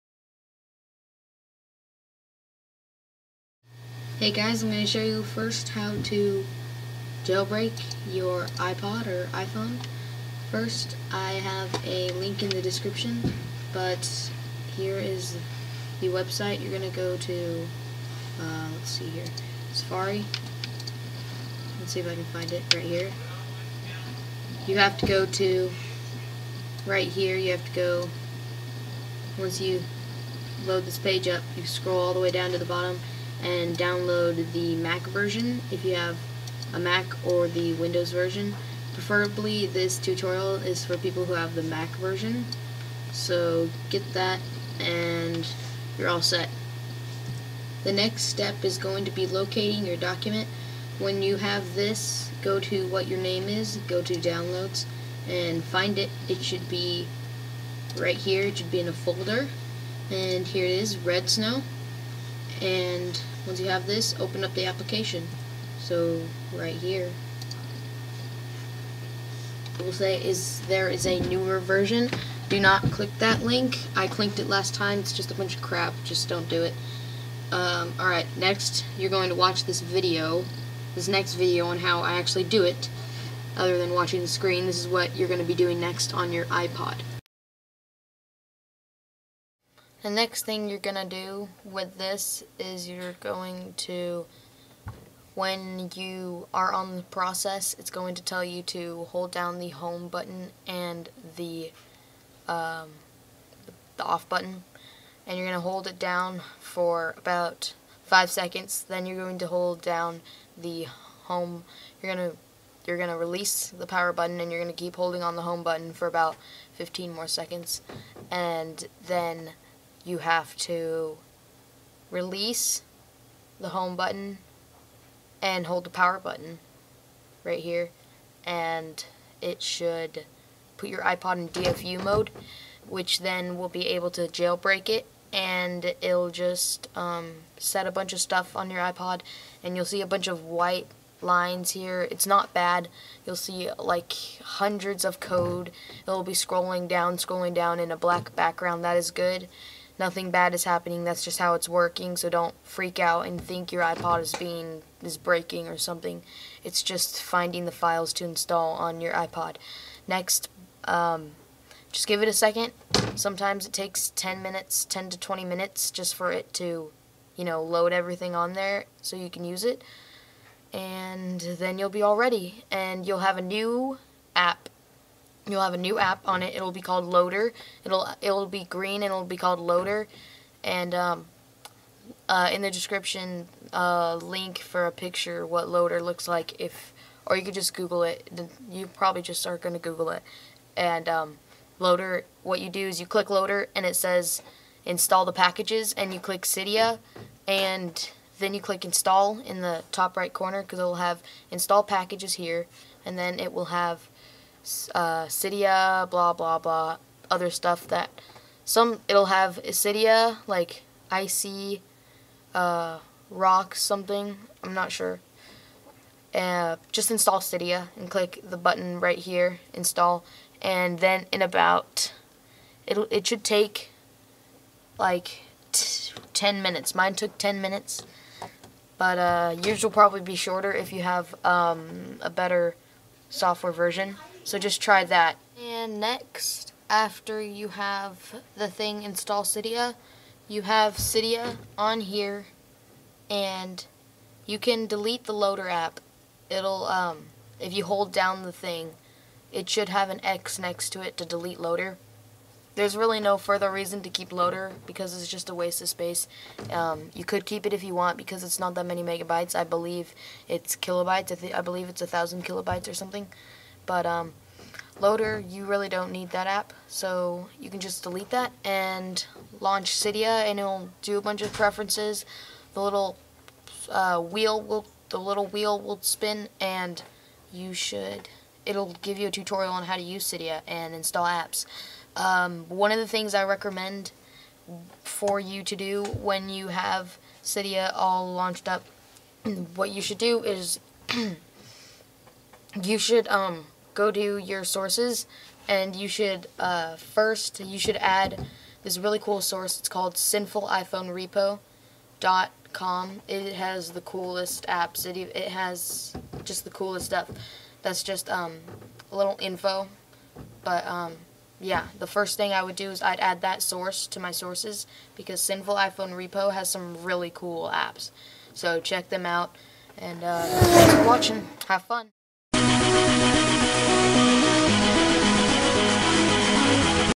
How To Jailbreak Ipod 5.0.1 EASY!!!
real easy tutorial just watch and you will have no confusion watsoever